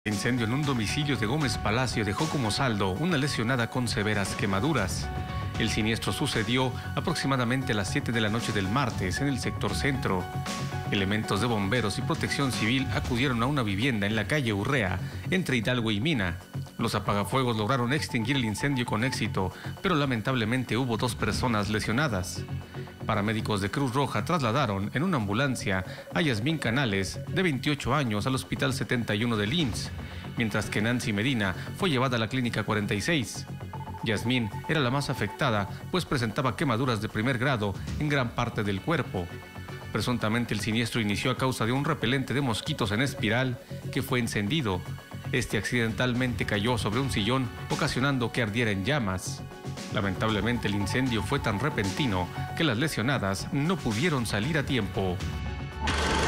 Dos mujeres lesionadas tras incendio; una muy grave
Un incendio de Gómez Palacio dejó como saldo una lesionada con severas quemaduras en la colonia Centro.